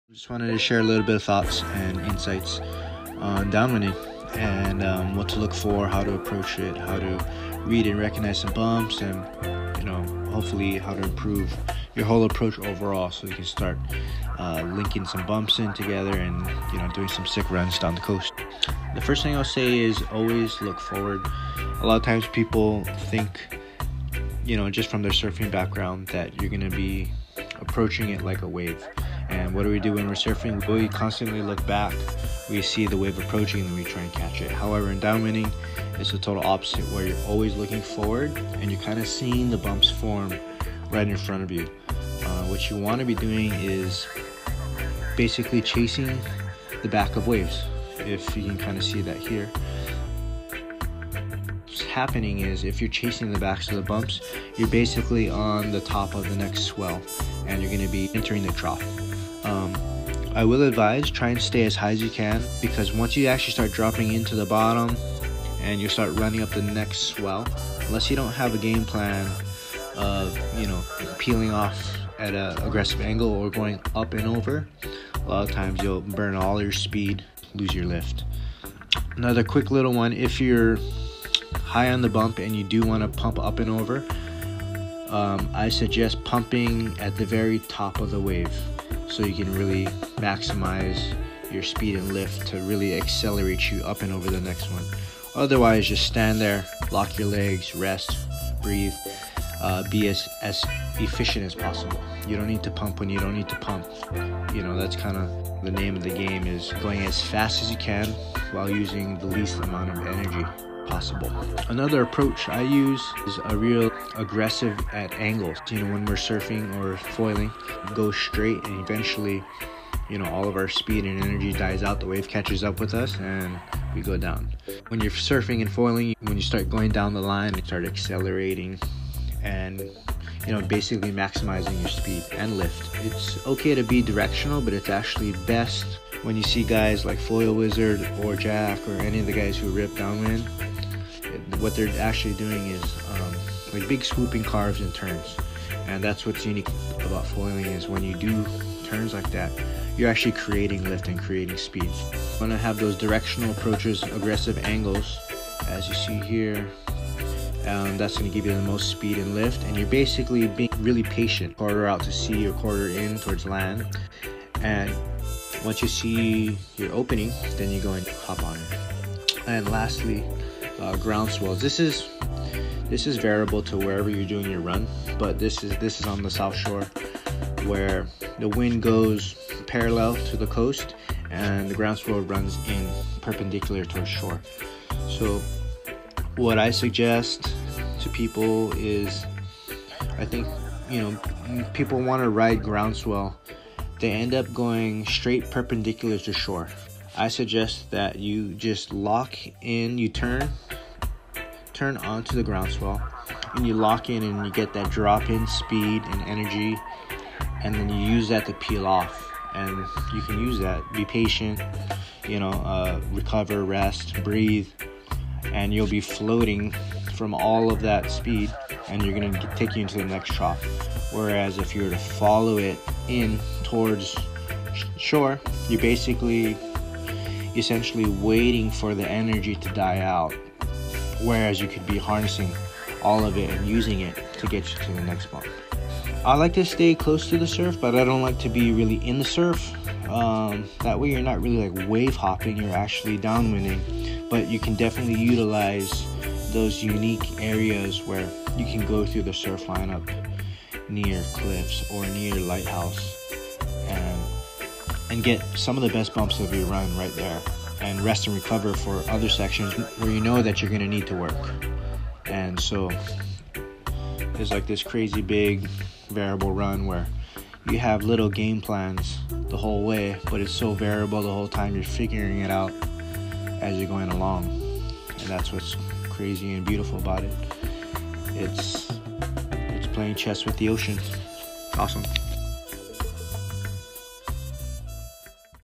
Introductory concepts and basics to Downwind Foiling
One of our first voiceover videos from a several years back!  Take a look and listen into some of the concepts that can hopefully help you along in the down wind foiling journey.  Footage and insight by Voyager foiler, enjoy